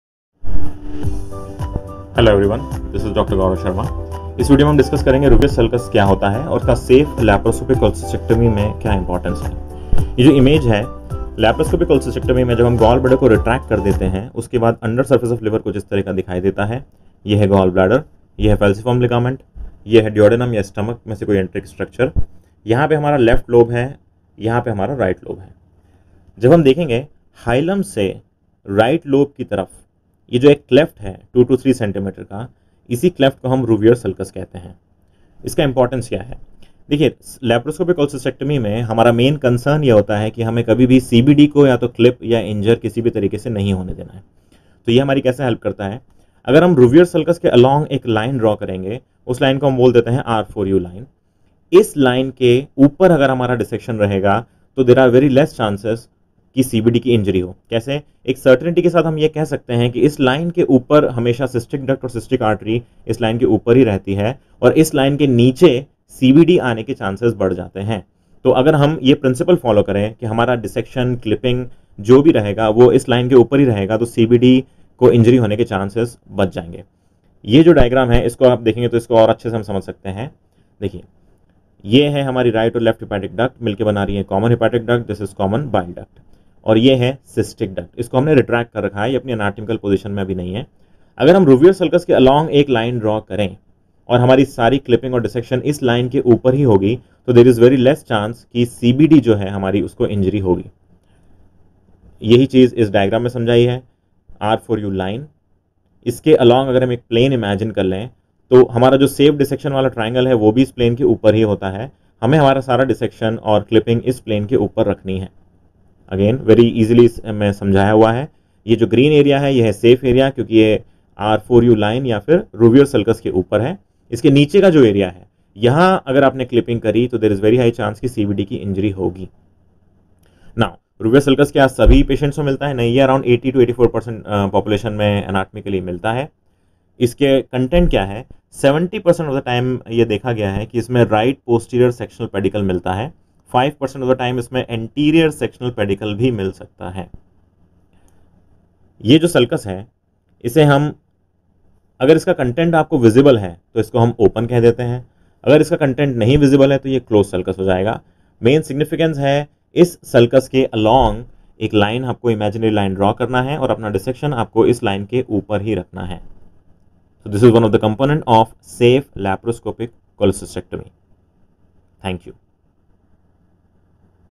what is Rouviere's Sulcus ??? its significance in safe Lap Cholecystectomy.(Hindi)
Rouviere's sulcus is a 2-3cm cleft running to the right of the liver hilum anterior to segment 1 and is usually containing the right portal triad or its branches. The sulcus indicates the plane of common

bile duct accurately.

represents a cleft, anterior to the traditionally described "Segment I of the liver", which has more recently been described by Couinaud13 as Segment IX

The most common advantage of identifying the sulcus is that the common bile duct lies

below it, and the cystic duct and artery above it.

Safe dissection zone. The R4U line is formed by Rouviere's sulcus (RS, four arrows) and runs from the base of liver segment IV to the hepatic ligament (R4U, red dotted line). This plane is the caudal border of the safe dissection zone. Above this plane is also the safe dissection triangle (7). Preparation above this plane ensures a safe distance to the main hepatic duct, its right branch, and first tion as well as the right hepatic artery. CD cystic duct, RHD right hepatic duct, PS Plane for save surgery

The branches of the right posterior sectional pedicle were found in the RS in 70% of the cases. In 5% of the livers, we also dissected a branch of the anterior sectional pedicle. Inside 25% of the RS, we found the vein of segment 6 (Figures 3(a) and 3(b)). In 18%, we found the inconstant cystical vein.